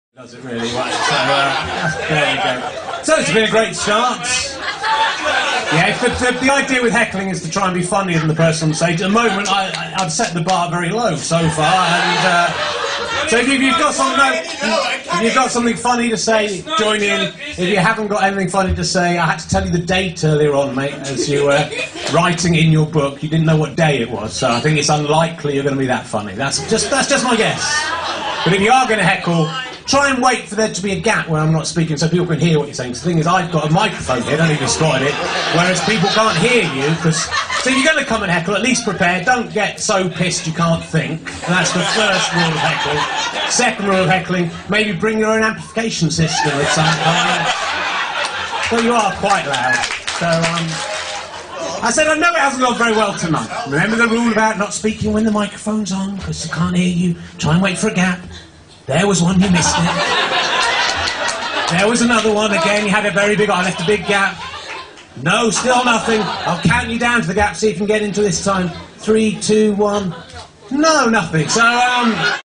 richard herring
Richard Herring deals with a heckler